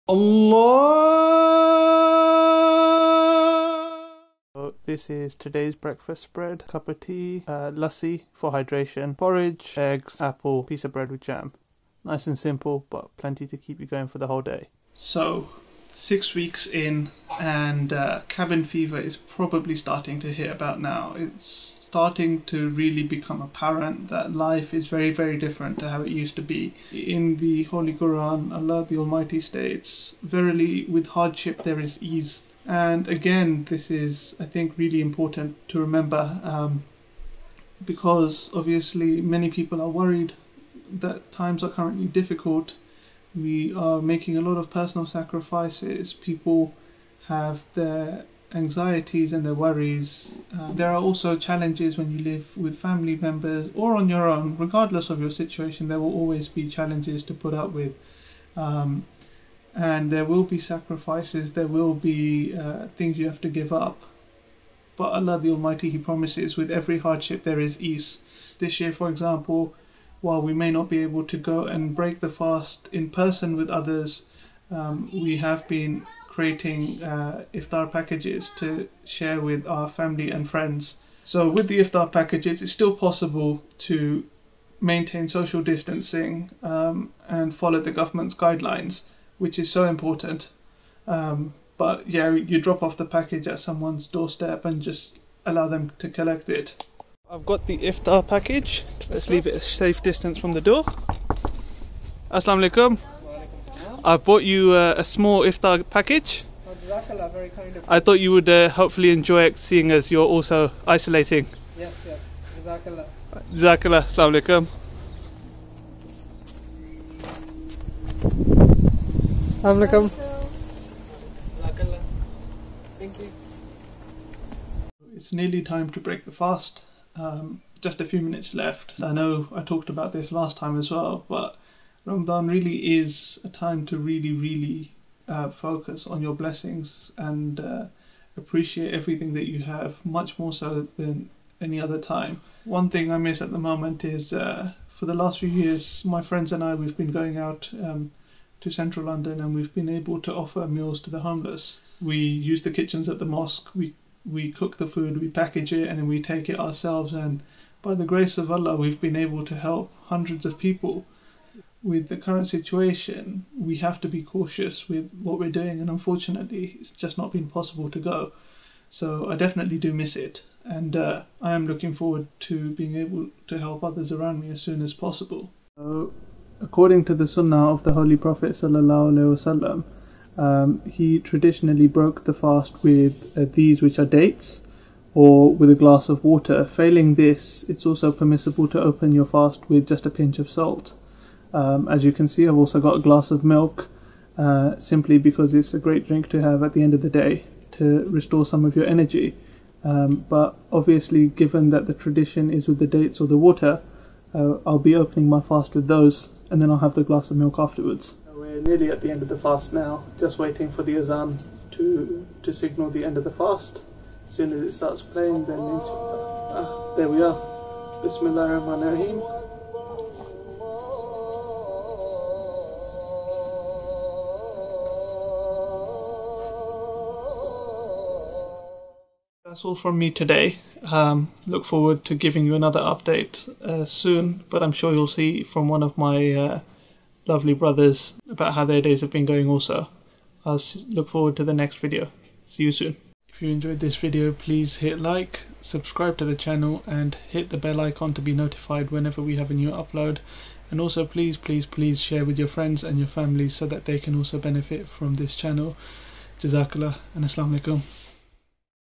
Ramadan Lockdown Video Diaries - E3
We join Noshe again today as he speaks about positivity during these unprecedented times.

Join us during this blessed month where we take a look into the daily lives of different Muslims, where their focus is and their approach to fasting and making use of their days during this pandemic.